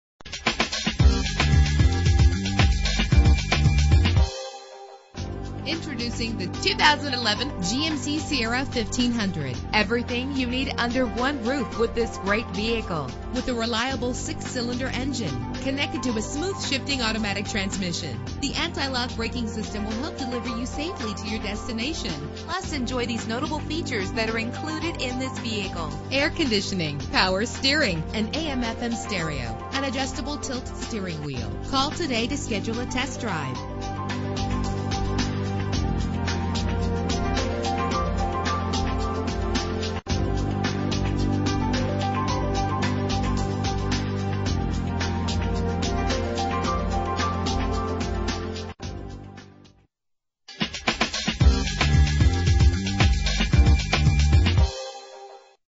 2011 GMC Sierra 1500 - St. Petersburg FL
Year: 2011
Make: GMC
Model: Sierra 1500
Trim: WT
Engine: 4.3 liter 6 cylinder 12 valve
Transmission: Automatic
Color: Silver
Mileage: 5
Address:
5301 34th st n